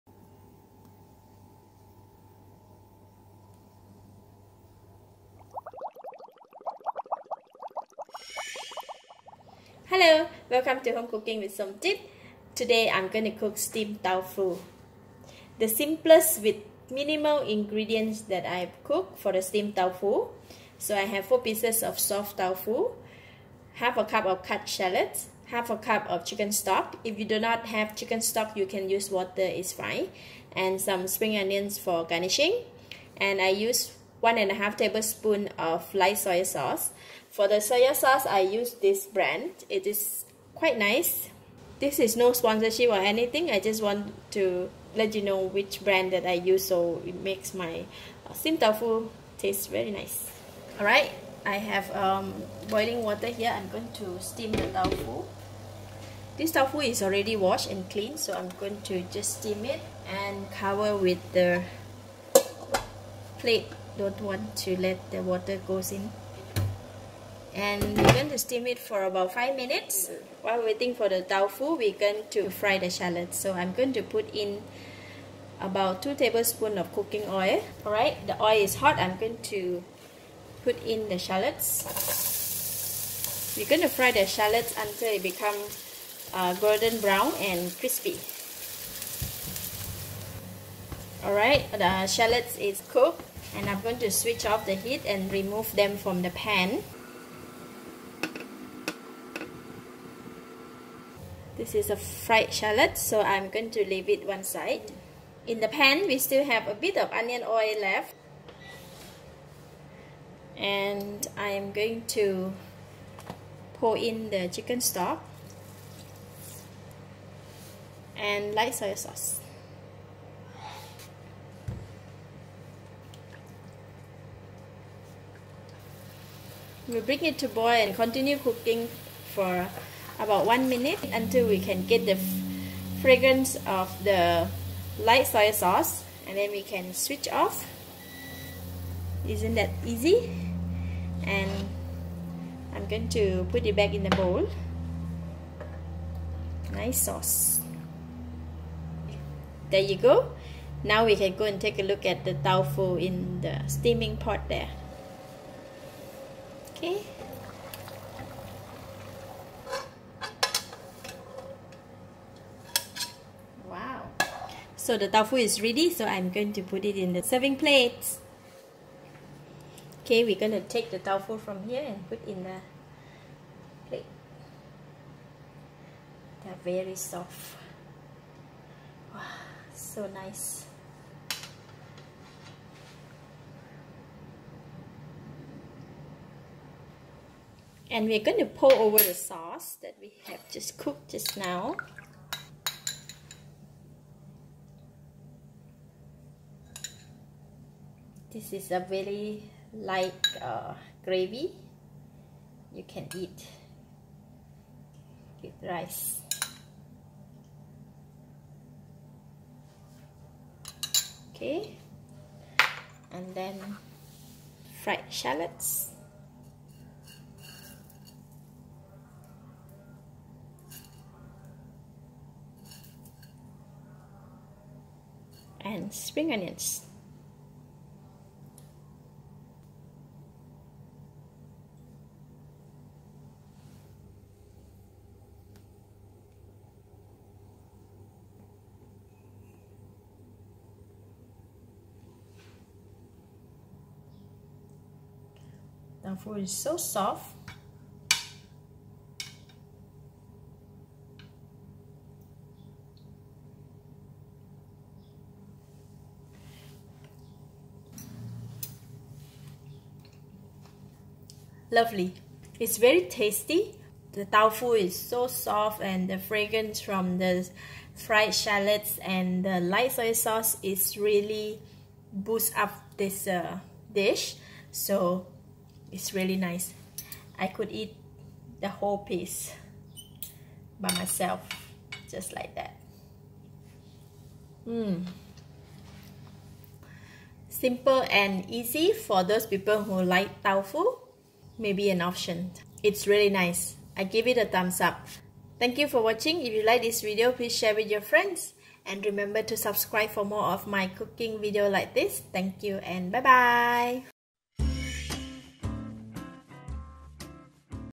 Simple Steamed Tofu in Soy Sauce (Soft Tofu Recipe)
Learn how to cook Steamed Tofu in Soy Sauce with Soft Tofu / Silken Tofu. The tofu is soft and smooth, making it nutritious and easy to eat. Pour the sauce over the steamed tofu and you have a dinner dish ready to go! Enjoy! Ingredients & Utensils ↓